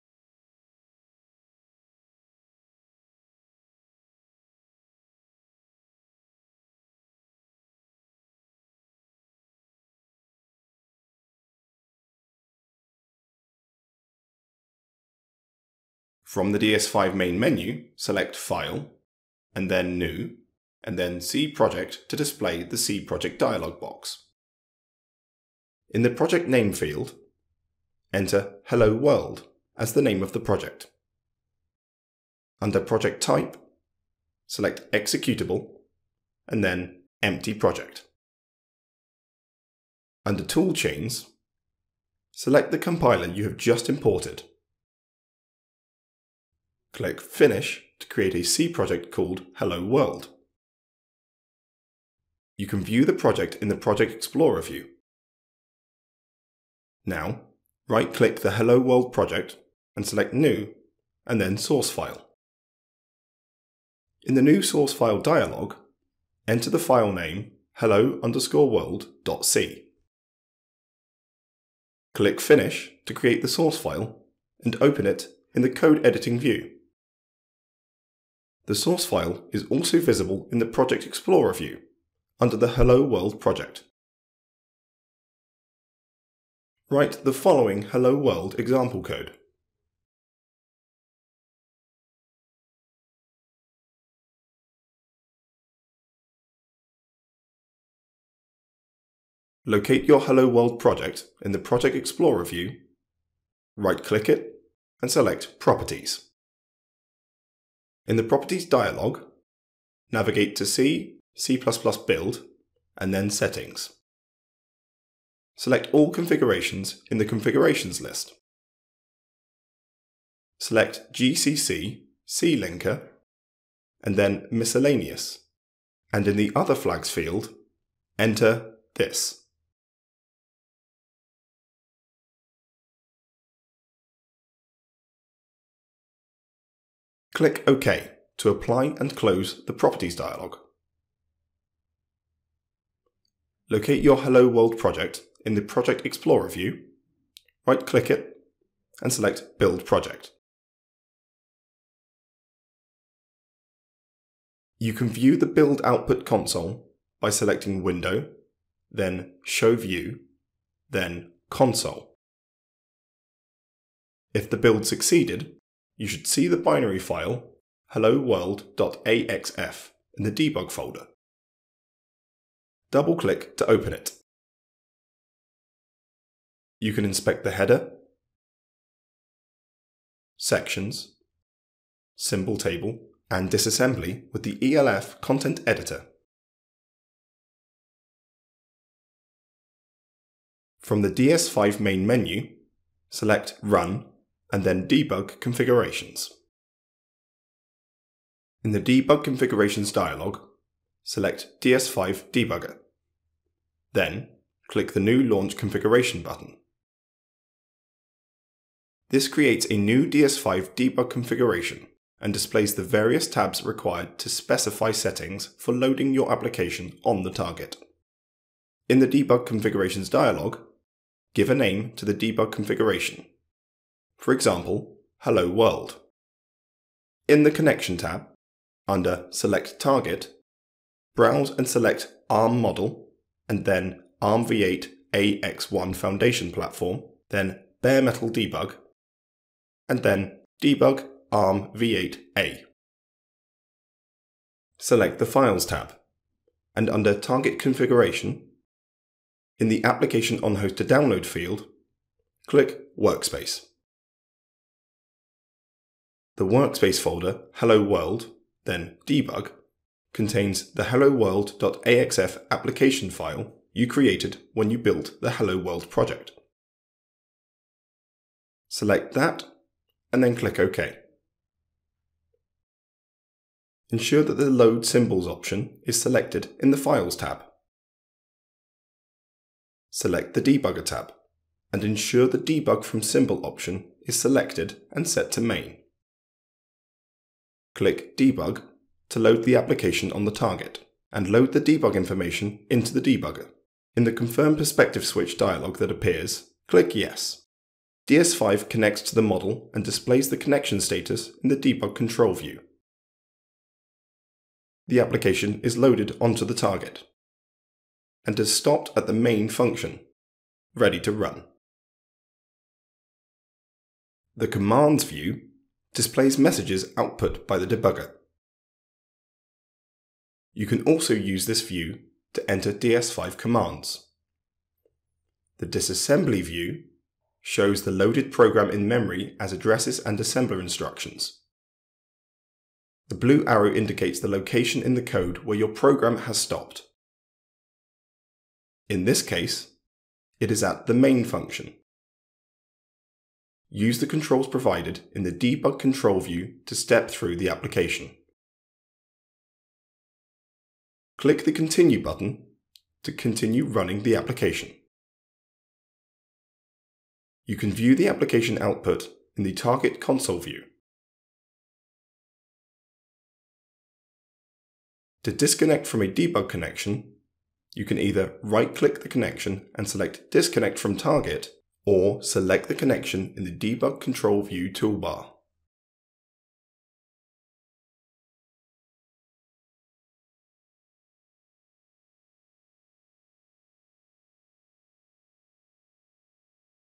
Part 3 - Create a Project for ARMv8 Model
This video series explains how to run applications on ARMv8 model using the ARM DS-5 Development Studio.